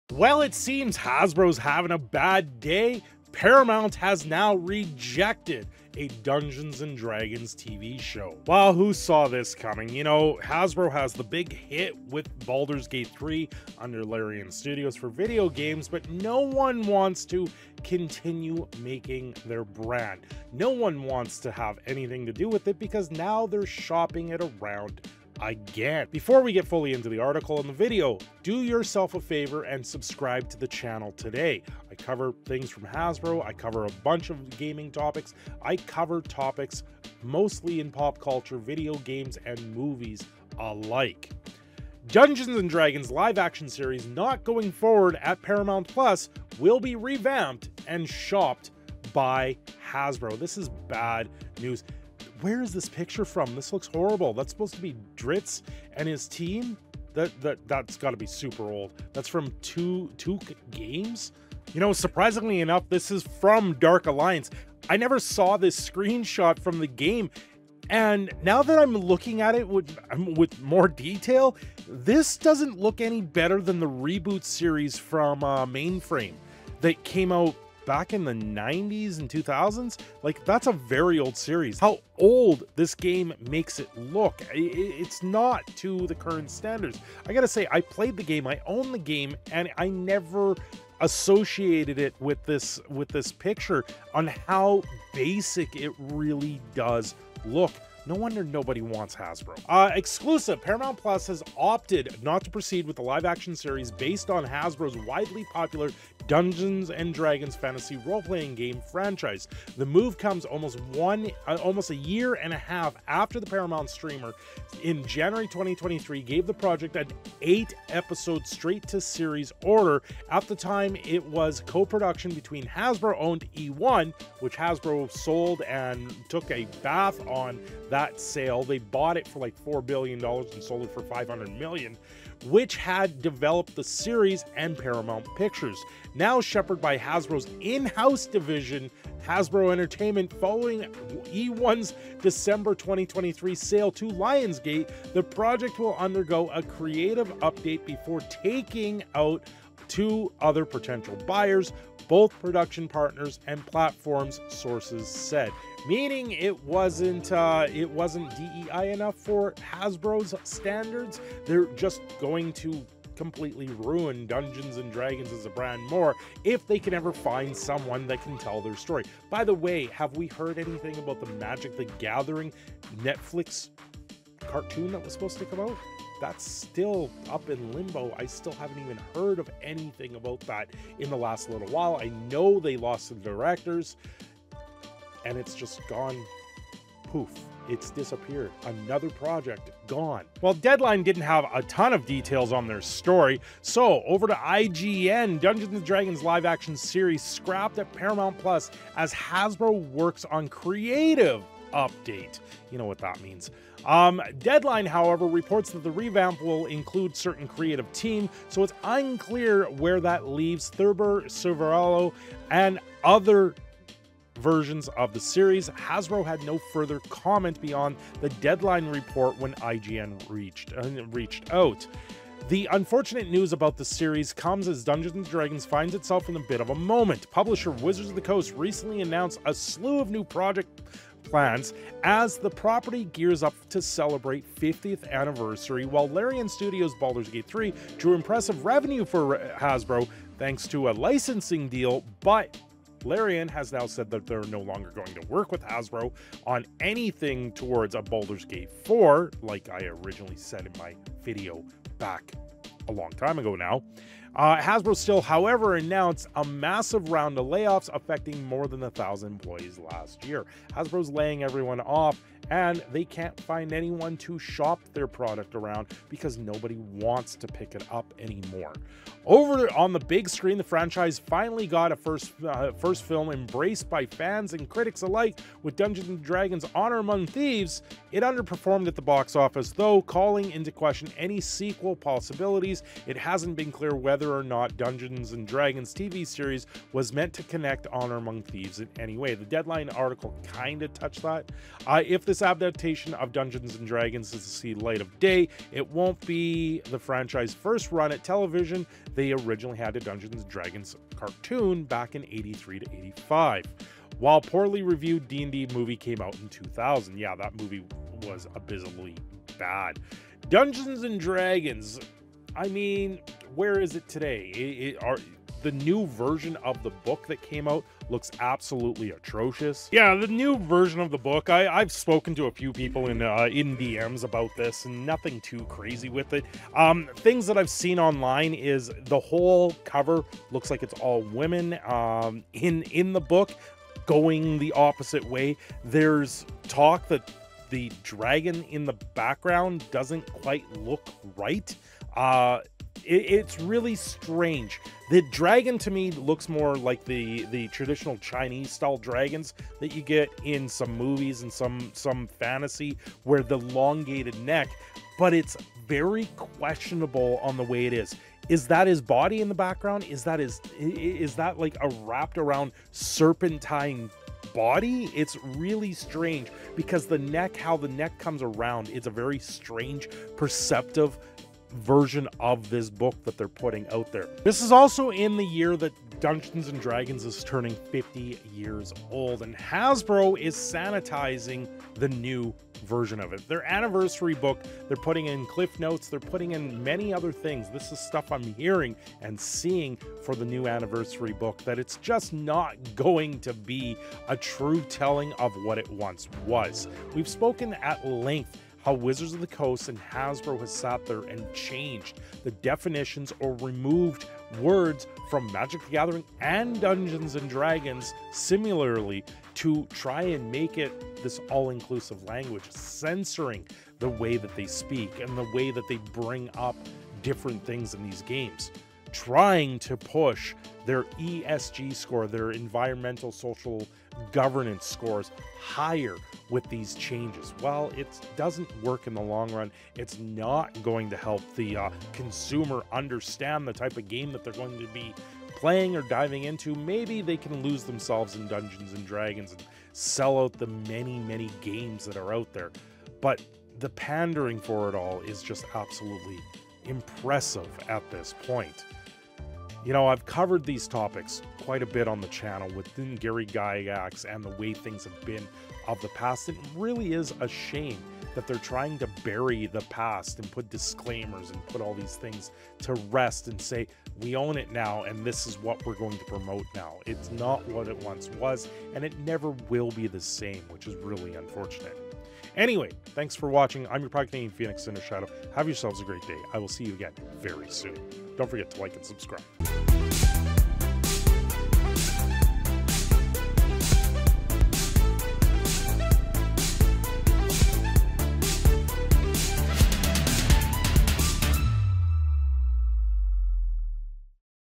Hasbro's Dungeon And Dragons Show Got CANCELLED
Paramount Plus cancelled Hasbro and WOTC Dungeon and Dragons show.